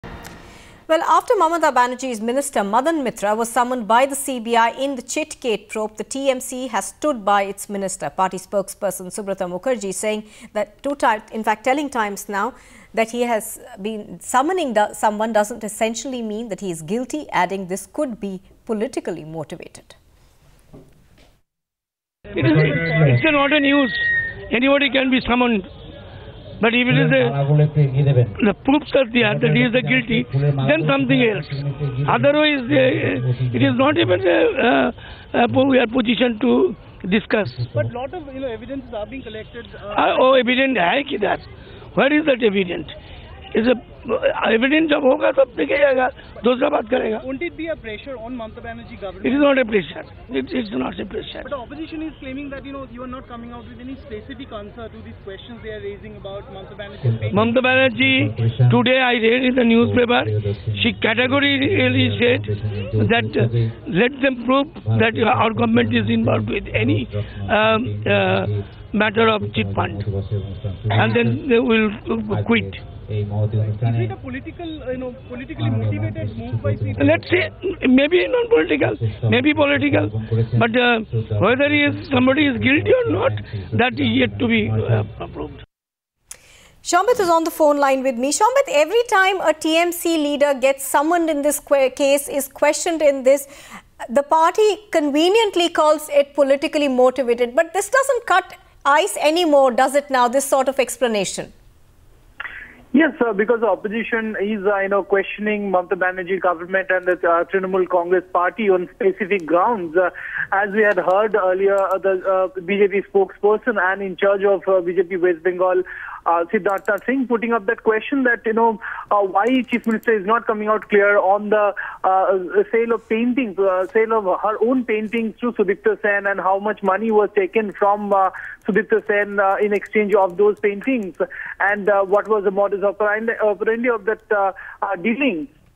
Under Pressure Mamata Defiant
The Chitfund heat is on West Bengal Chief Minister, Mamata Banerjee. As pressure mounts on her, she is ready to step down if the chitfund links are proved. Meanwhile, the BJP has sharpened its attack on the TMC and asking Mamata to answer the questions around the multi-crore chirtfund scam. Despite coming in the line of attack, a defiant TMC continues to defend itself reiterating the party chief's stance.